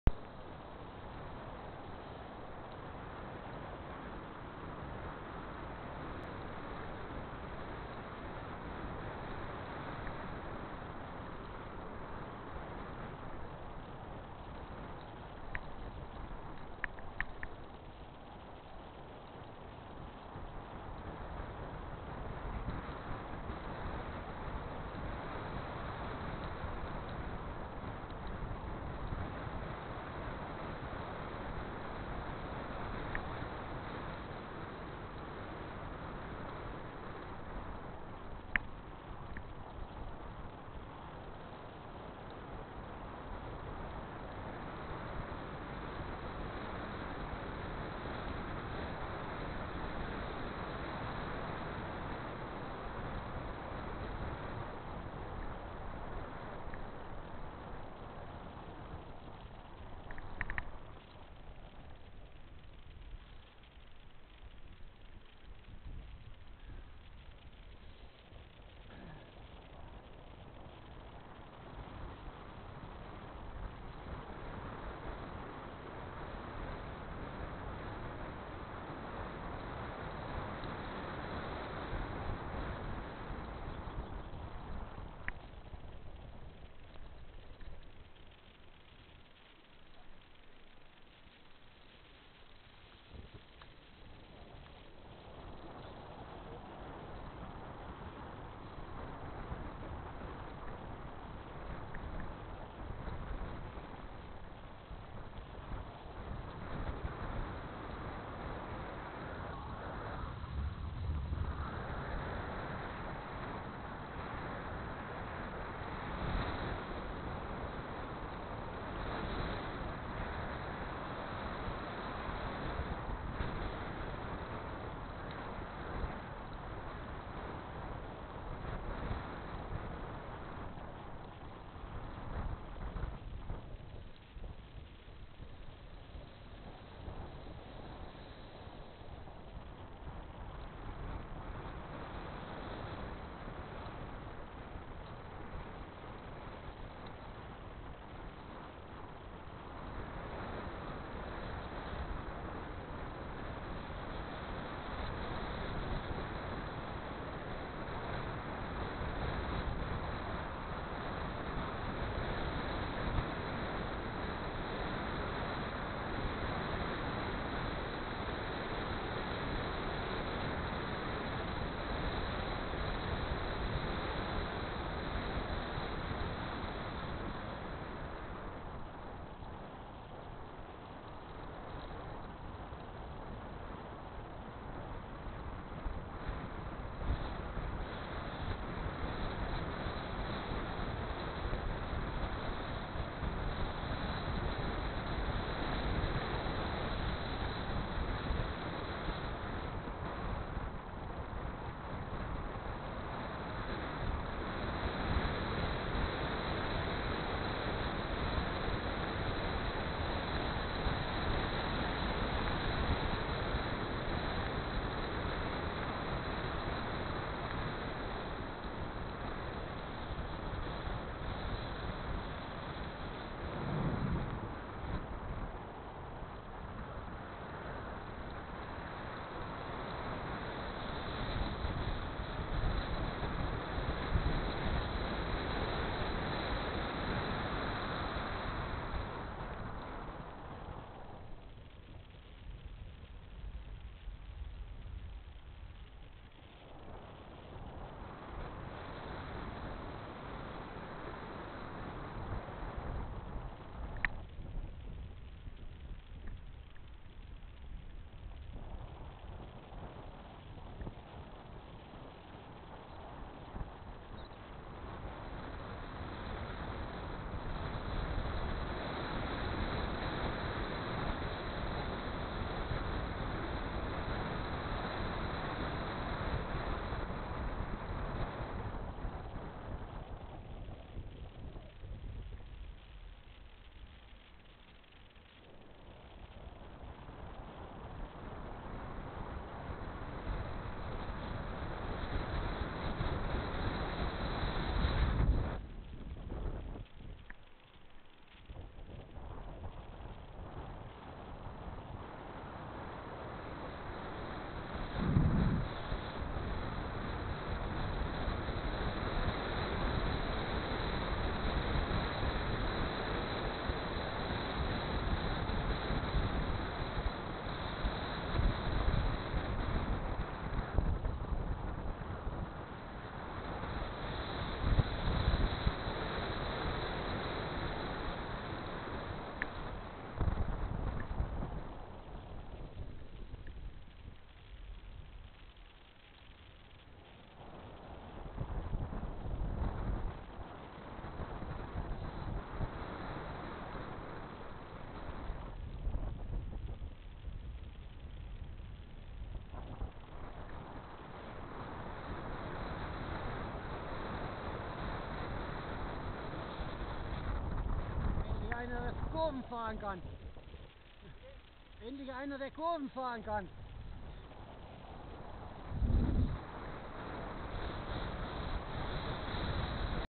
Dreilaendergiro 2009 #11-1 Descending Umbrailpass 2 of 3
Dreilaendergiro Dreiländergiro 2009 Nauders
Descending Umbrailpass to St Maria (CH)
Distance covered 4.2km, max speed 57,0km/h
starting altitude 2015m, finish altitude 1680m
Oregon scientific ATC5K action camera